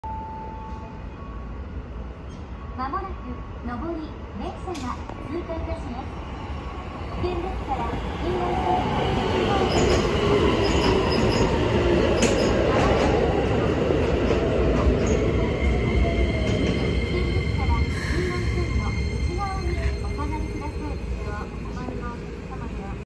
京急大森海岸駅 1000形普通神奈川新町行きが停車
京急大森海岸駅で撮りました。1000形普通神奈川新町行きが停車します。